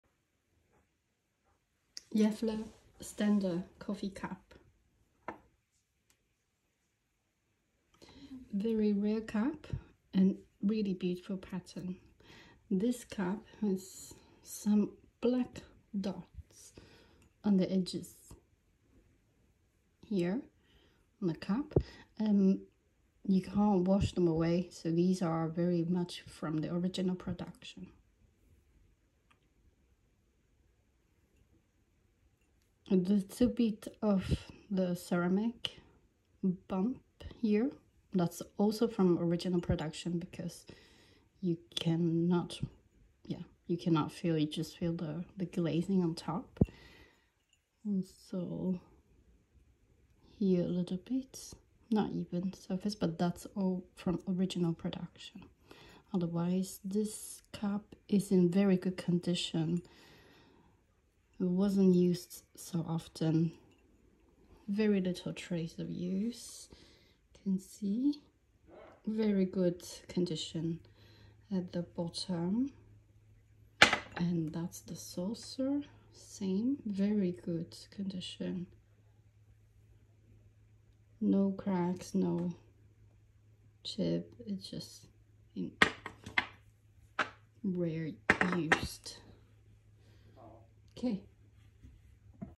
Upsala Ekeby Gefle Standard coffee cup set
Rare decor Gefle Standard in orange & red colours by Berit Ternell / Eugen Trost
Antique from 1960s by Upsala Ekeby Gefle.